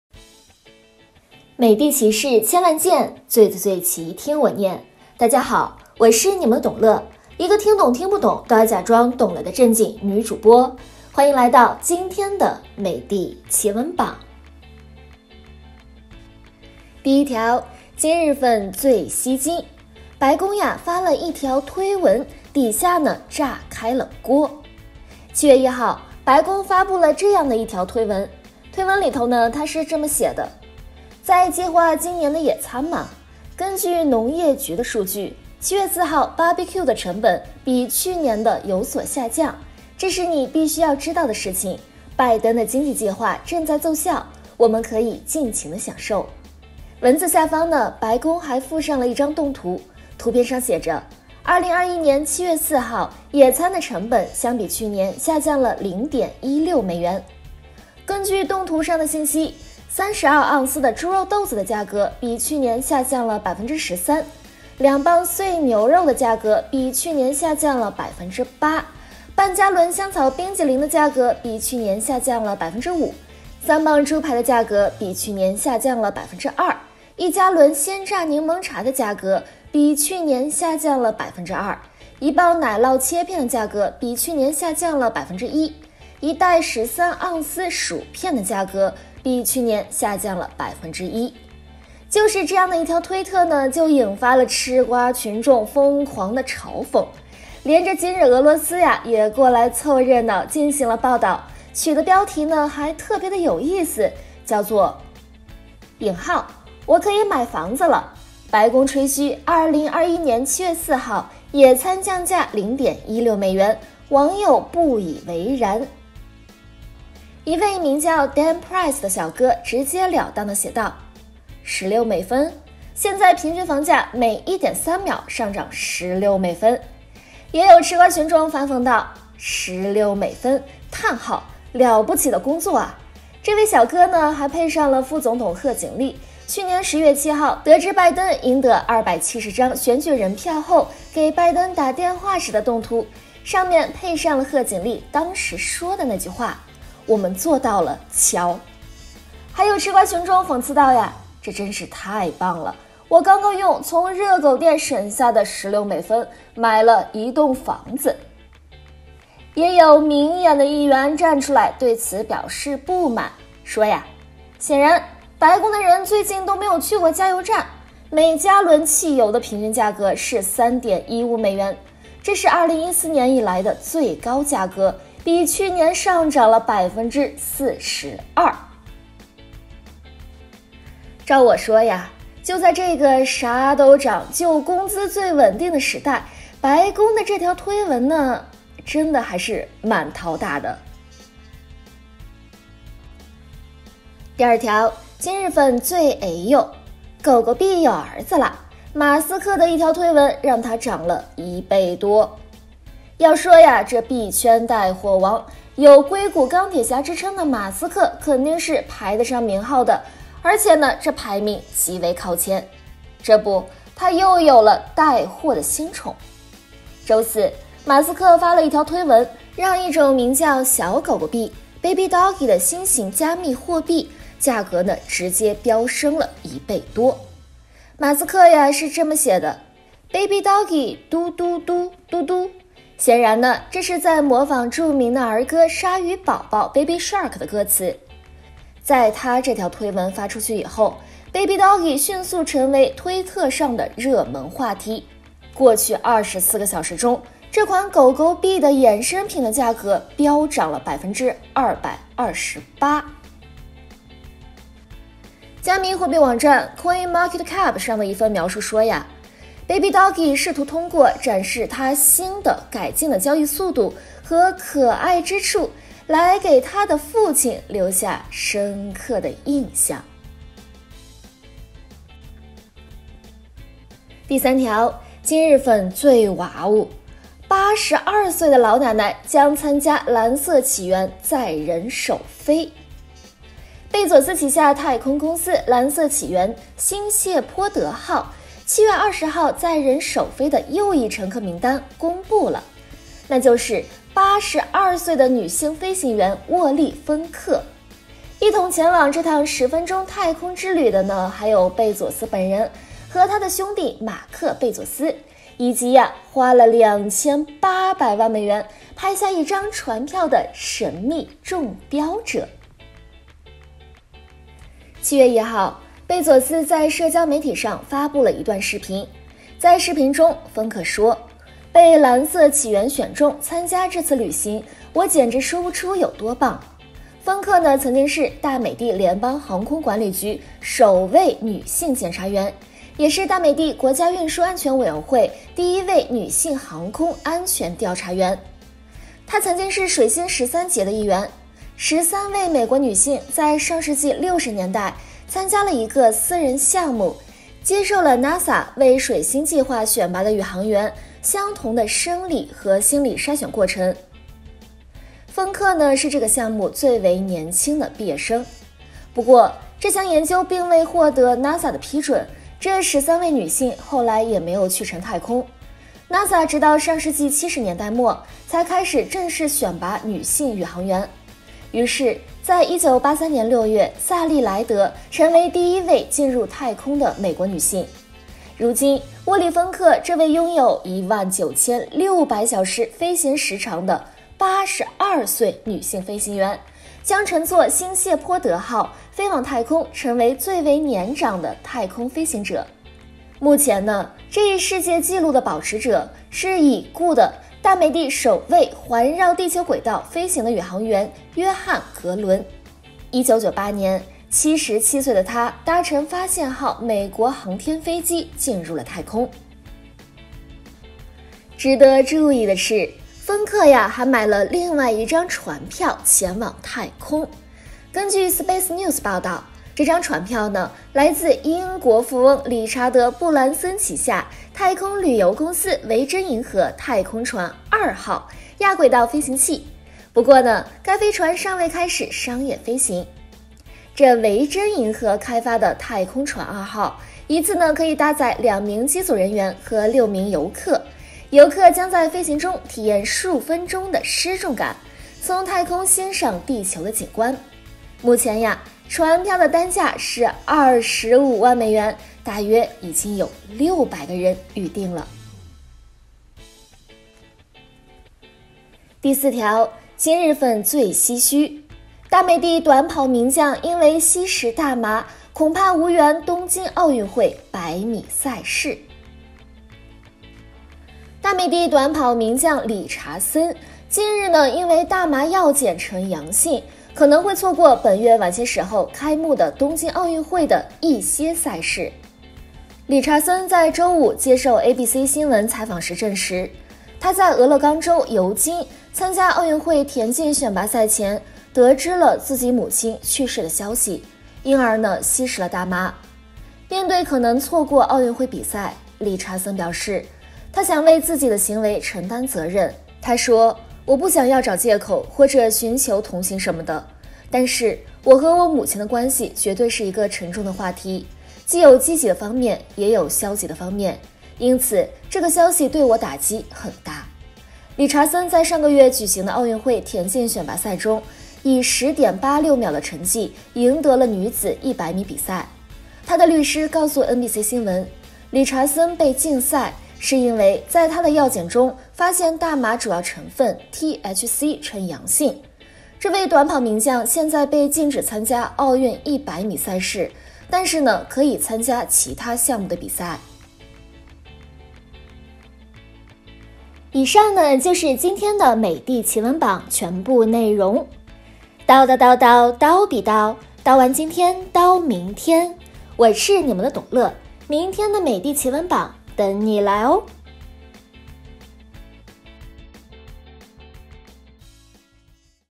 【美帝奇闻榜】白宫发了一条推文，底下炸开锅；狗狗币有儿子啦！马斯克一条推文让它涨了一倍多…
推文是这么写的：“在计划今年的野餐吗？……根据农业局的数据，7月4日的BBQ成本比去年有所下降。这是你必须知道的事情……拜登的经济计划正在奏效。我们都可以尽情享受。”